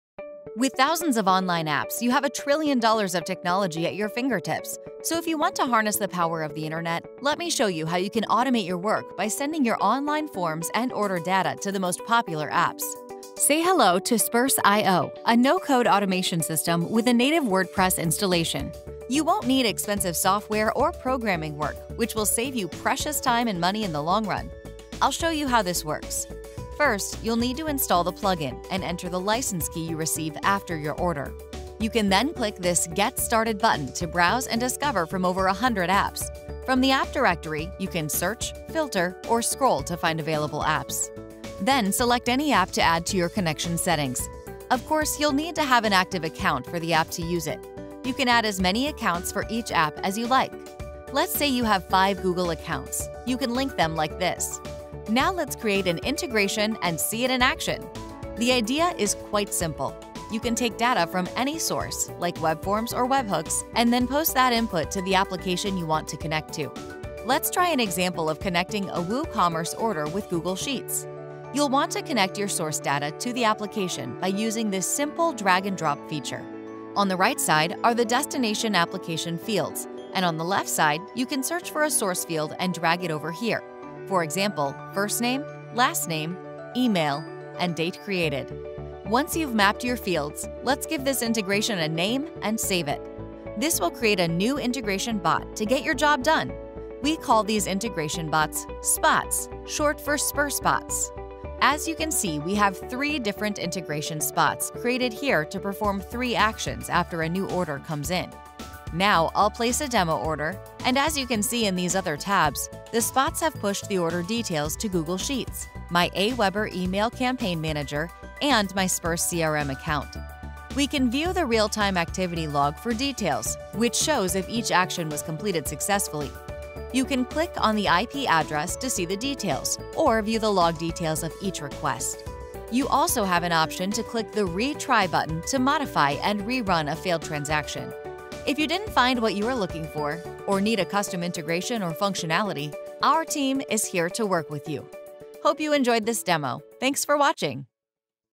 Sperse.io Automate Hub - App Integrations Platform Demo
Automate Hub is a discovery and integration WordPress Plugin for SaaS apps. Users can seamlessly connect with over 100 apps, from web forms to popular CRMs, Email Service Providers, Marketing Automation, SMS and Webinar platforms, and Unified Communication tools through the Sperse IO Automate Hub, using easy drag-and-drop field-mapping and zero code!